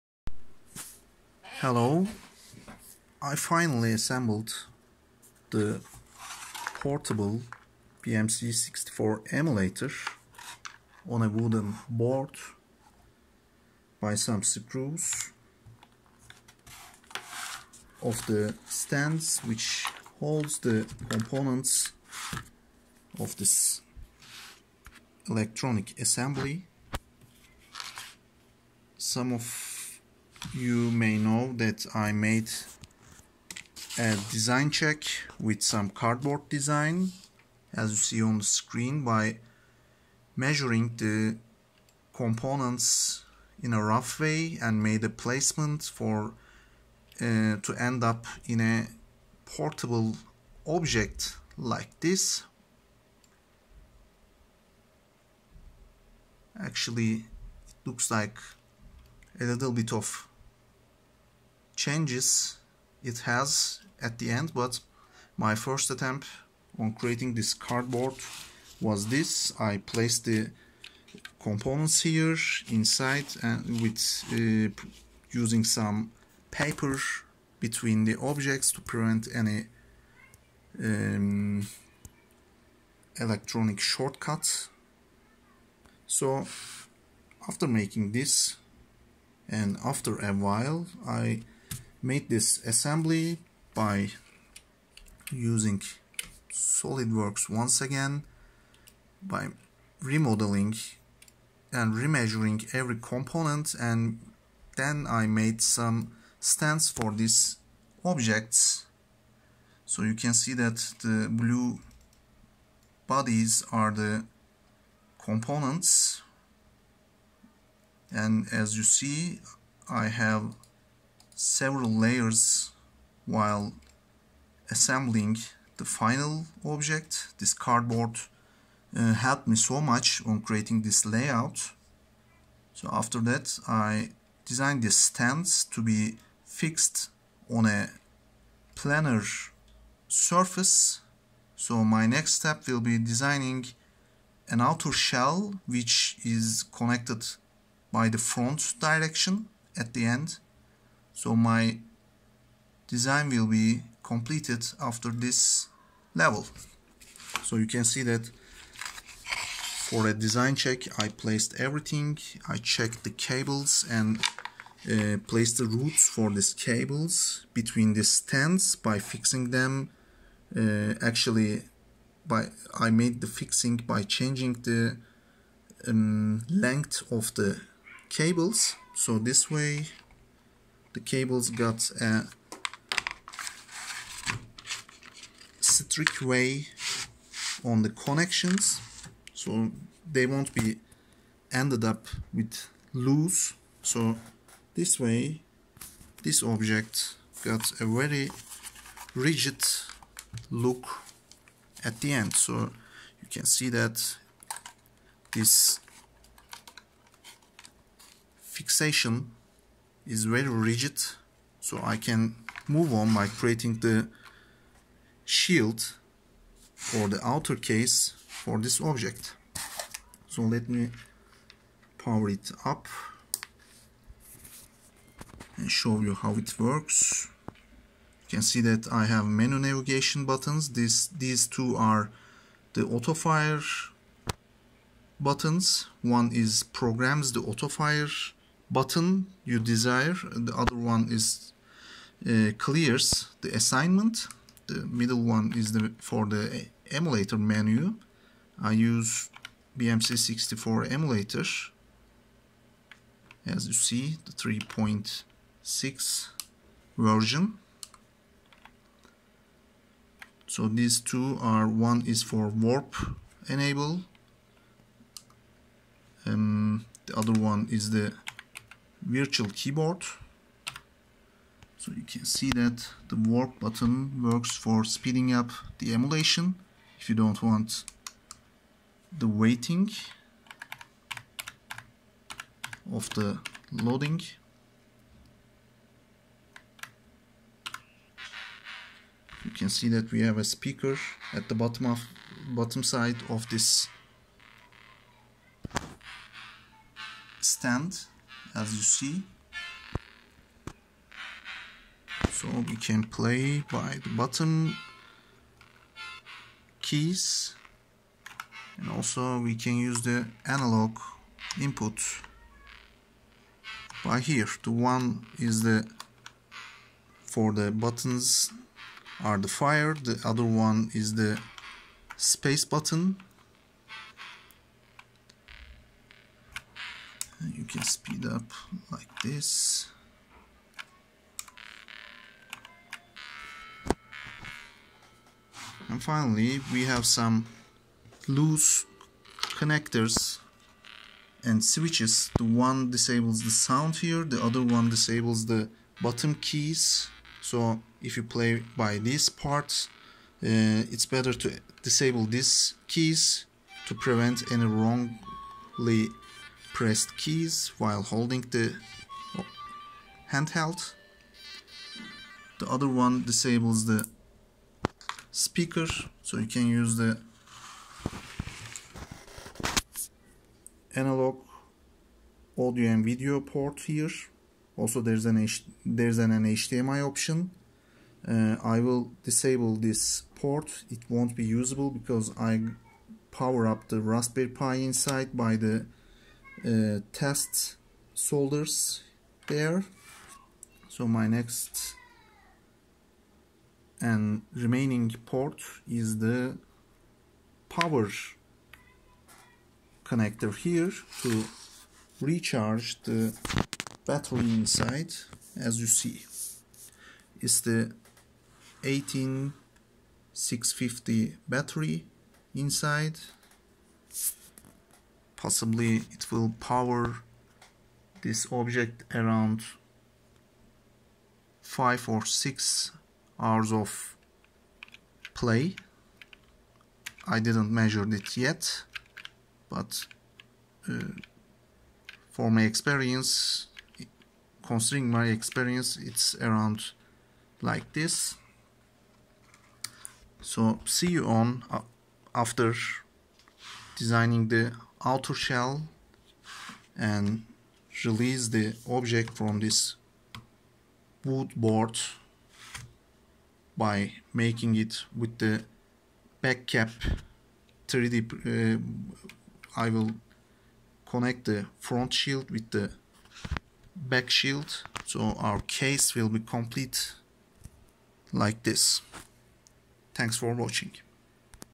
Portable Commodore 64 with BMC64 Emulator
I made a progress on my portable C64 device.